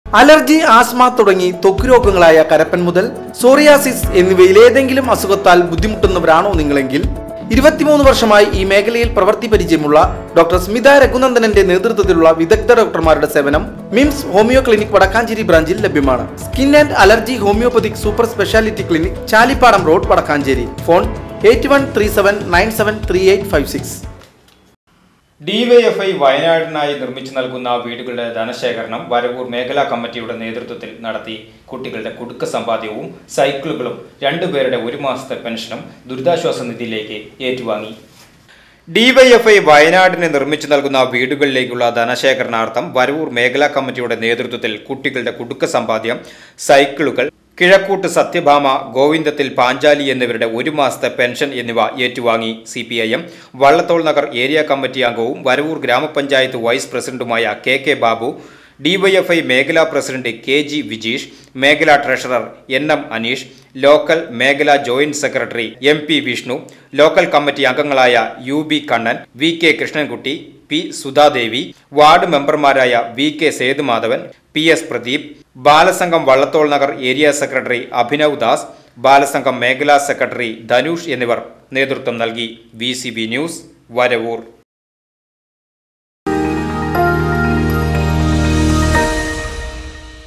DYFI വയനാടിനായി നിർമ്മിച്ച് നൽകുന്ന വീടുകളിലേക്കുള്ള ധന ശേഖരണം വരവൂരിൽ നടന്നു
VCV NEWS

VCV NEWS CHANNEL
Wadakkanchery, Thrissur